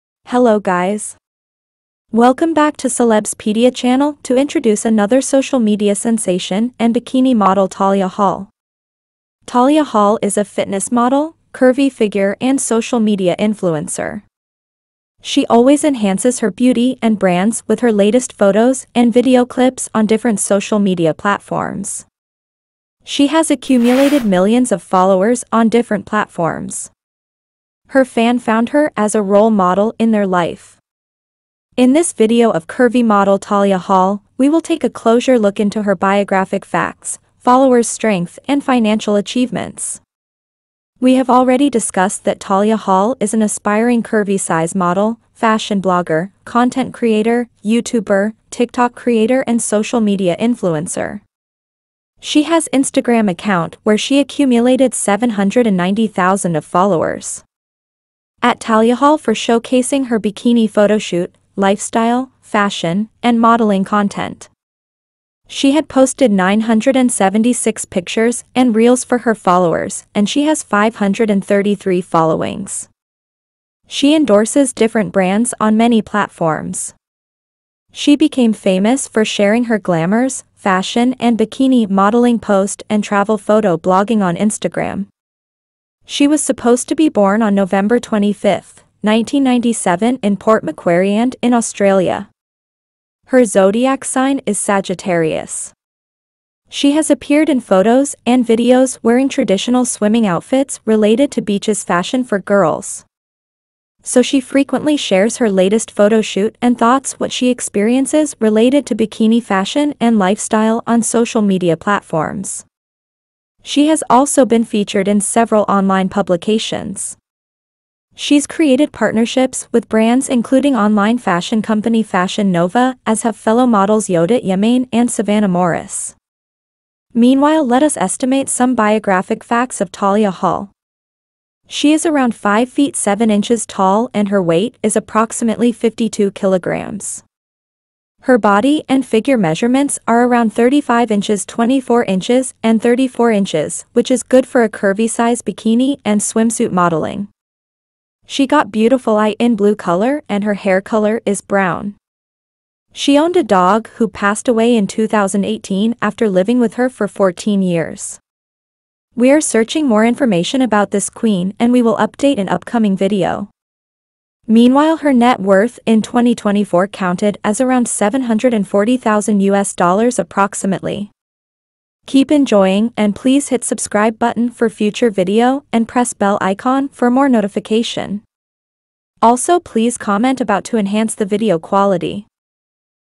Tahlia Hall ✅ Wiki, Biography, Brand Ambassador, Age, Height, Weight, Lifestyle, Facts
Hello guys,
Welcome back to CelebsPedia channel to introduce another social media sensation and bikini model Tahlia Hall.  Tahlia Hall is a Fitness Model, Curvy figure and Social Media Influencer. She always enhances her beauty and brands with her latest photos and video clips on different social media platforms. She has accumulated millions of followers on different platforms. Her fan found her as a role model in their life. In this video of curvy model Tahlia Hall, we will take a closure look into her biographic facts, followers’ strength and financial achievements.
We have already discussed that Tahlia Hall is an aspiring curvy size model, fashion blogger, content creator, Youtuber, tiktok creator and social media influencer.

She has Instagram account where she accumulated 790 thousand of followers
@tahliaahall for showcasing her bikini photoshoot, lifestyle, fashion, and modeling content. She had posted 976 pictures and reels for her followers and she has 533 followings. She endorses different brands on many platforms.
She became famous for sharing her glamour’s, fashion and bikini modeling post and travel photo blogging on Instagram.

She was supposed to be born on November 25, 1997 in Port Macquarieand in Australia. Her zodiac sign is Sagittarius. She has appeared in photos and videos wearing traditional swimming outfits related to beaches fashion for girls.
So she frequently shares her latest photoshoot and thoughts what she experiences related to bikini fashion and lifestyle on social media platforms. She has also been featured in several online publications. She's created partnerships with brands including online fashion company Fashion Nova as have fellow models Yodit Yemane and Savannah Morris.
 Meanwhile Let us estimate some biographic facts of Tahlia Hall.  She is around 5 feet 7 inches tall and her weight is approximately 52 kilograms.  Her body and Figure Measurements are around 35 inches 24 inches and 34 inches which is good for a curvy size bikini and swimsuit modeling.  She got beautiful eye in blue color and her Hair Color is brown. She owned a dog who passed away in 2018 after living with her for 14 years.
We are searching more information about this queen and we will update in upcoming video.
Meanwhile her net worth in 2024 counted as around 740 thousand US dollars approximately.
Also please comment about to enhance the video quality.